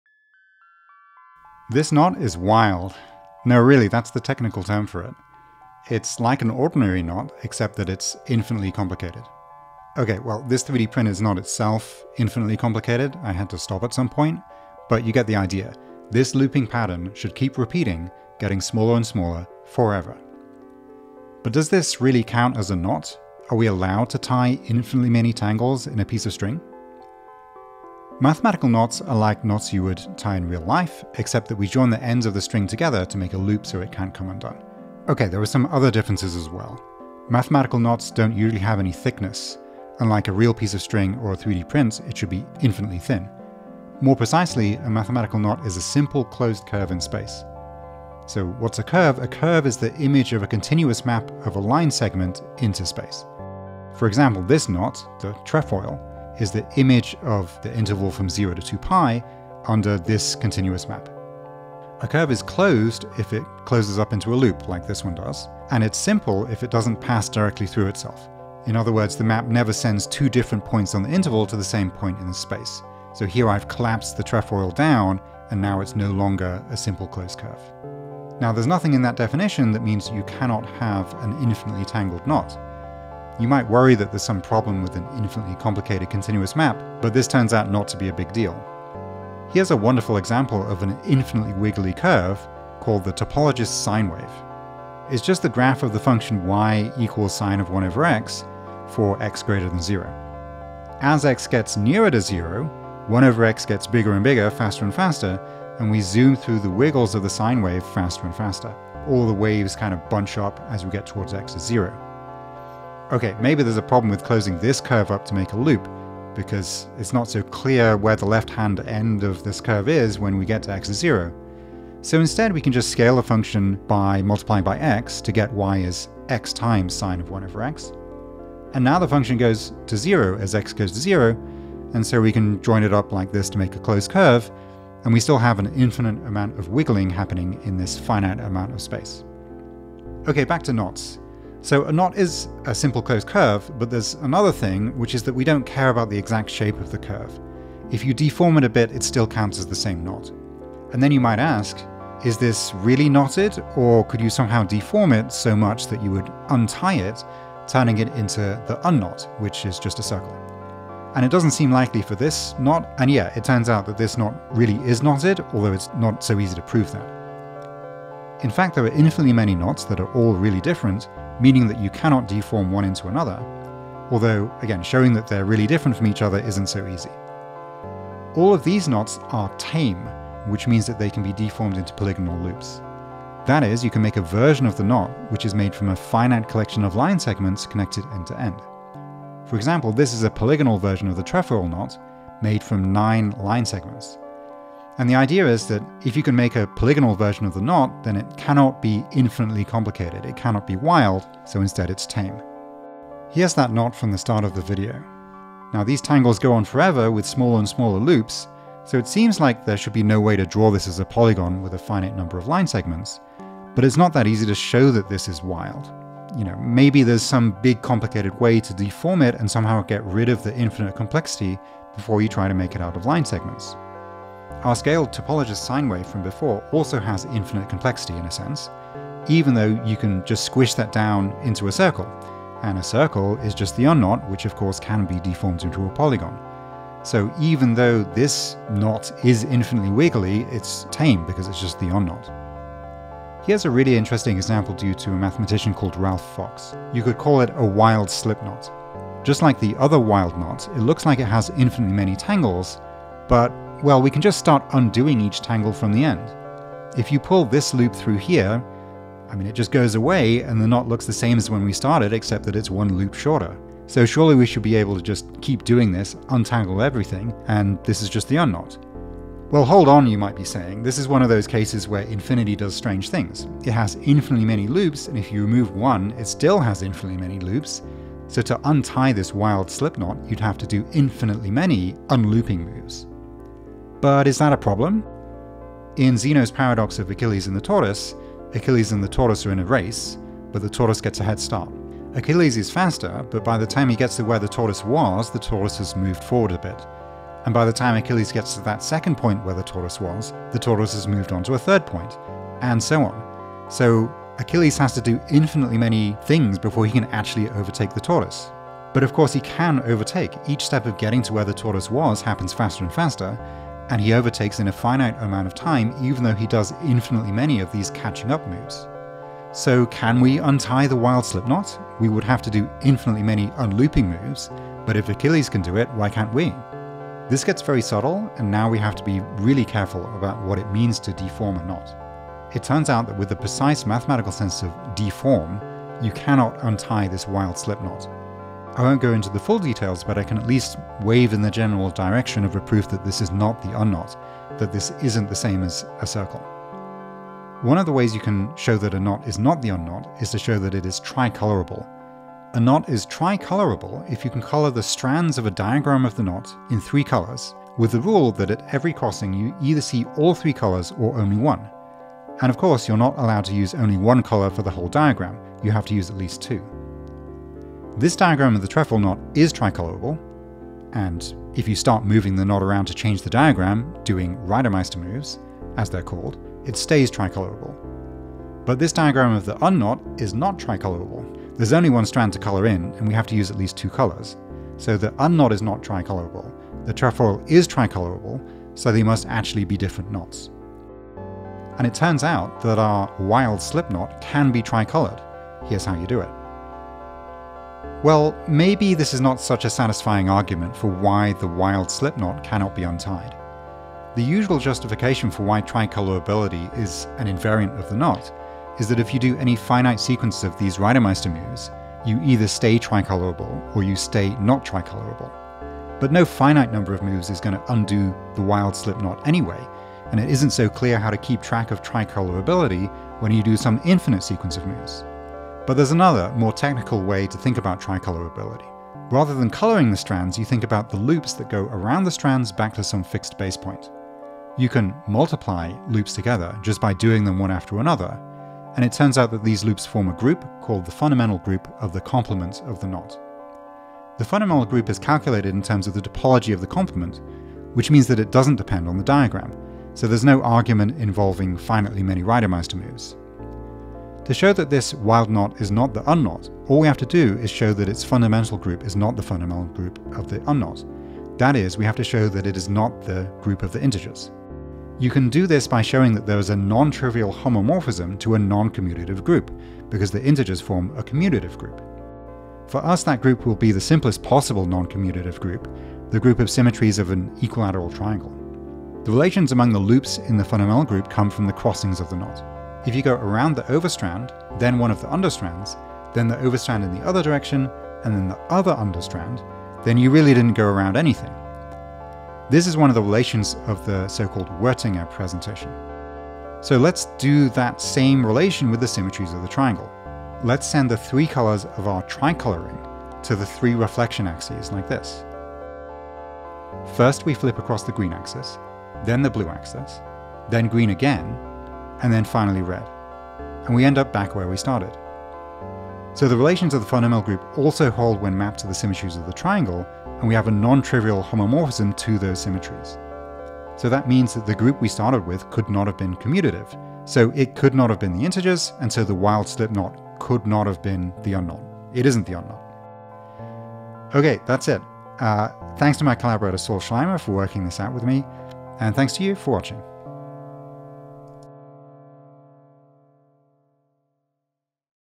Wild knots
A video about some infinitely complicated, fractal knots.

The paper by Ralph Fox is:
"A remarkable simple closed curve", Ann. of Math., (2) 50, 264-265.

Hsin-Po Wang came up with this awesome animation of undoing the wild slipknot (which is, of course, impossible)